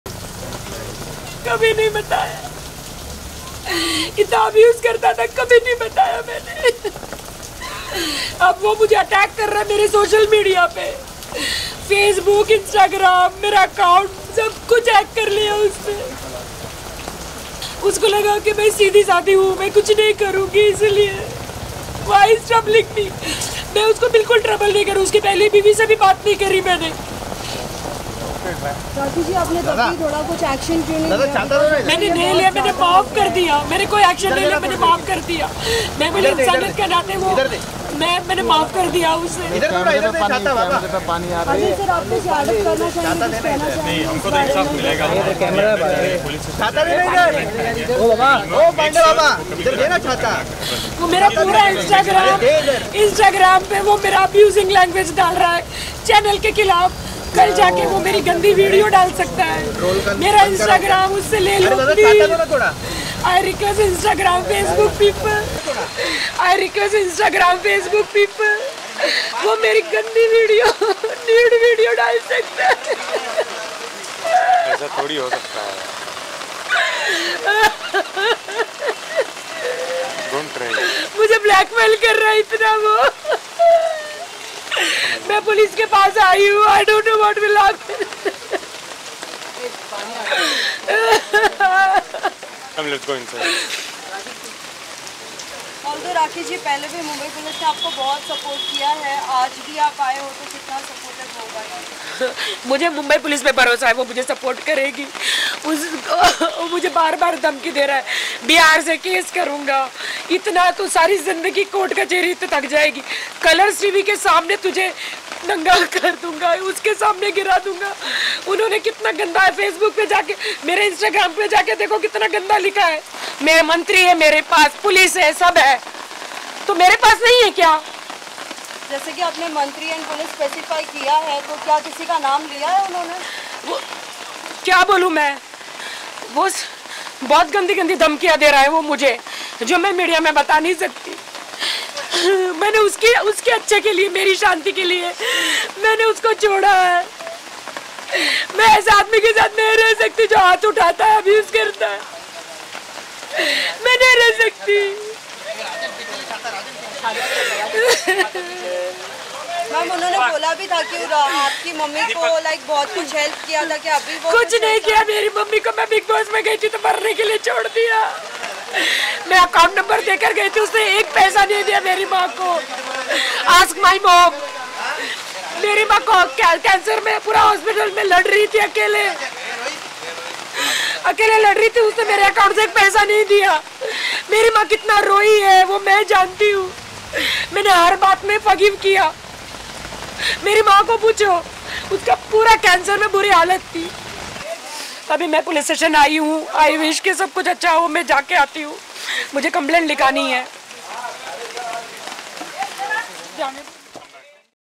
Rakhi Sawant breakdown in Tears after her Shocking loss in her Life, Files FIR Against Ritesh
Rakhi Sawant breakdown into Tears after her Shocking loss in her Life, Files FIR Against Ritesh